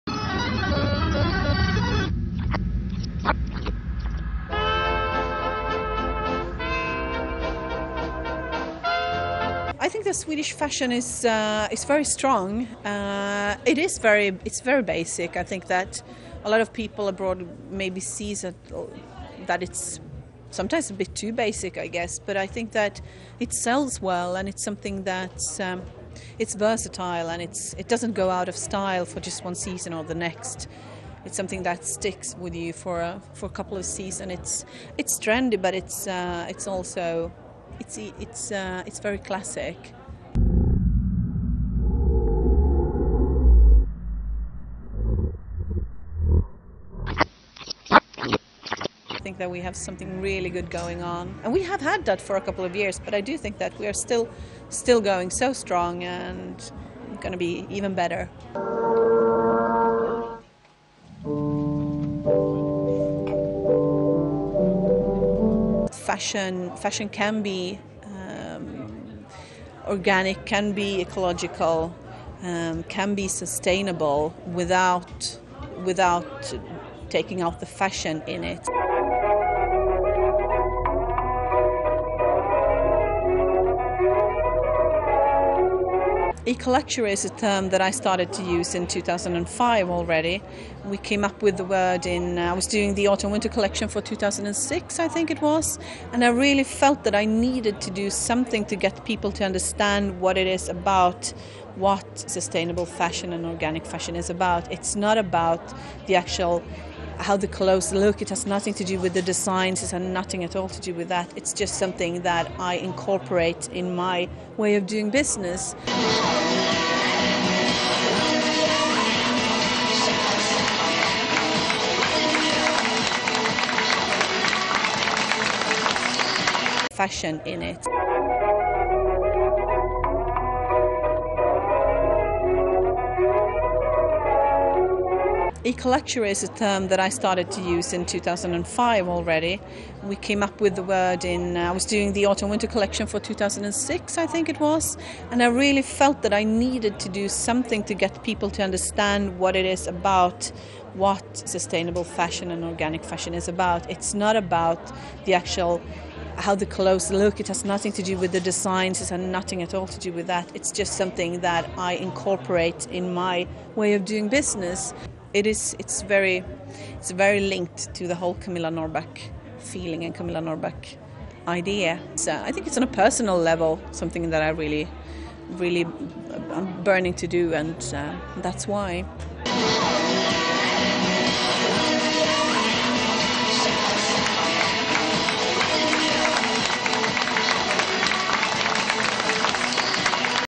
Camilla Norrback at Stockholm Fashion Week by Berns
Interview with Camilla Norrback, designer Camilla Norrback and general views fashion show